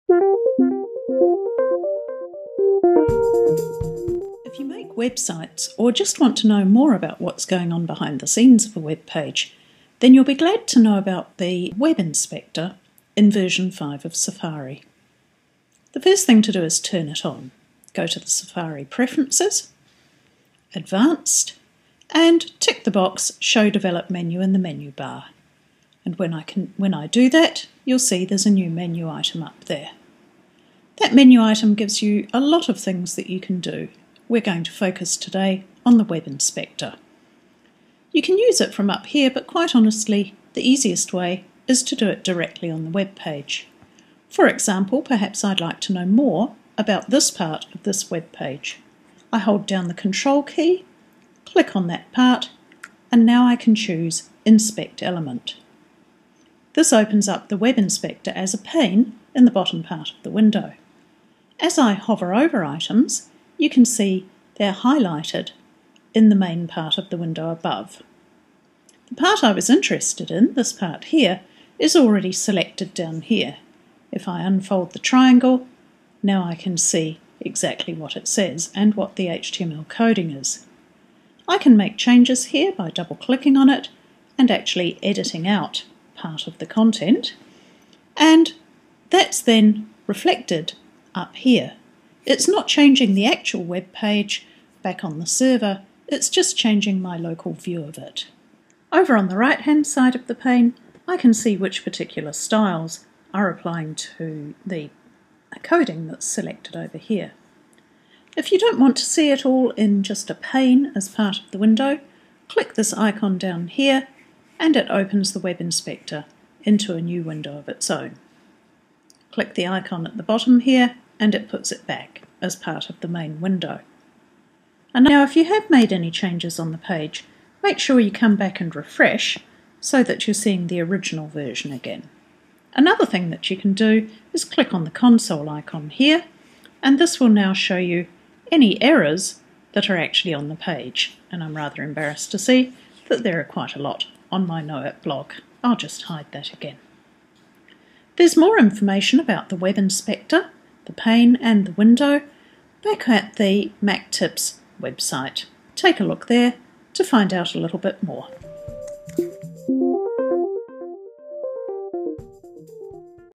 Use Safari 5's Web Inspector - a gift for web developers
Use Safari 5's Web Inspector - a gift for web developers, Mac Tip #444, 07 July 2010: If you're a web developer working with Safari 5 there's an easy way to see exactly what HTML and CSS is being used for a portion of a web page. Here's how to enable and use the Web Inspector. Read the Tip and make comments at